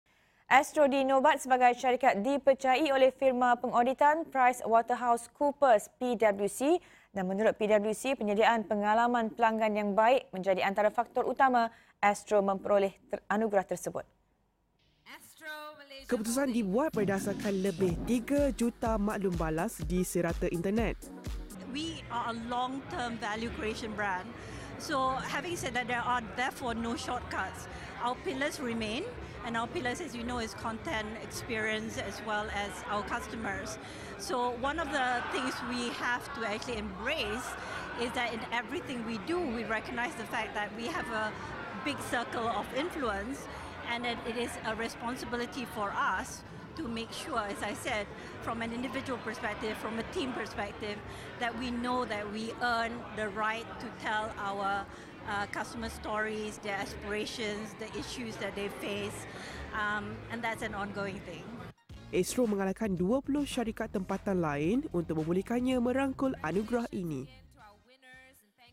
Astro rangkul anugerah khas kepercayaan PWC
Astro dinobat sebagai "syarikat dipercayai" oleh Firma Pengauditan, Price Waterhouse Coopers (PWC).
Menurut pwc, penyediaan pengalaman pelanggan yang baik menjadi antara faktor utama Astro memperoleh anugerah tersebut.